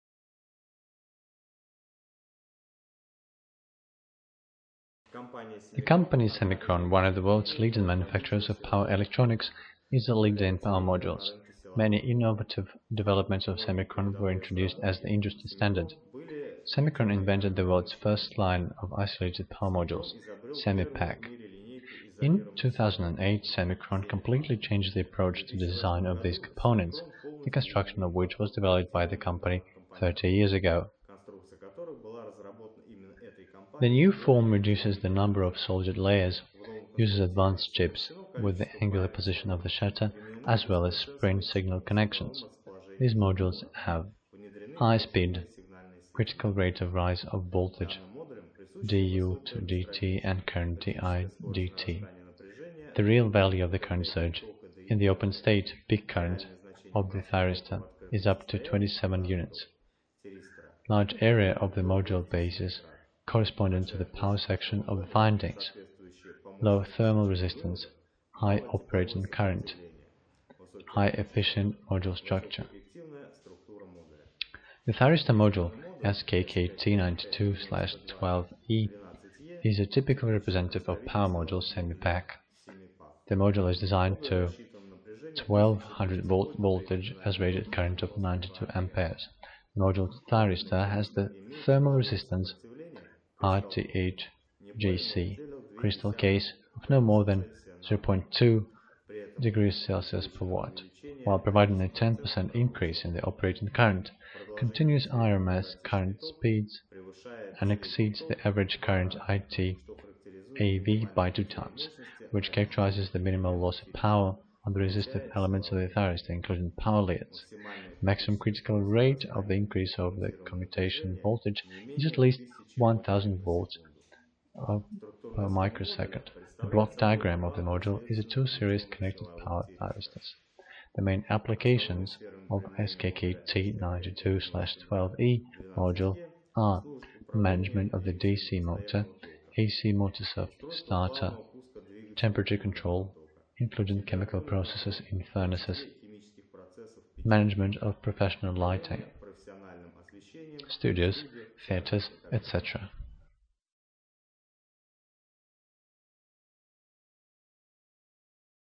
SCR unit SKKT92-12E
SCR unit SKKT92-12E       The company SEMIKRON, one of the world's leading manufacturers of power electronics, is a leader in power modules. Many innovative developments of SEMIKRON were introduced as the industry standard.       SEMIKRON invented the world's first line of isolated power modules - SEMIPACK®.       In 2008, SEMIKRON completely changed the approach to the design of these components, the construction of which was developed by this company 30 years ago.       The new form reduces the number of soldered layers, uses advanced chips with the angular position of the shutter, as well as spring signal connections. These modules have:       high speed (critical rate of rise of voltage dU/dt and current di/dt),       the real value of the current surge in the open state (peak current) of the thyristor is up to 27 units,       large area of ​​the module basis, corresponding to the power section of the findings,       low thermal resistance (high operating current),       high efficient module structure.       The thyristor module SKKT92/12E is a typical representative of power modules SEMIPACK. The module is designed to 1200V voltage at a rated current of 92A. The module thyristor has the thermal resistance Rth(j-c) "crystal-case" of no more than 0.2°C/W, while providing a 10% increase in the operating current. Continuous RMS current exceeds the average current IT(AV) by 2 times, which characterizes the minimal loss of power on the resistive elements of the thyristor, including power leads.       Maximum critical rate of increase of the commutation voltage is at least 1000V/microsecond. The block diagram of the module is a two series-connected power thyristors.       The main applications of SKKT92/12E module are: management of the DC motor, AC motor soft starter, temperature control (including chemical processes in furnaces), management of professional lighting (studios, theaters), etc.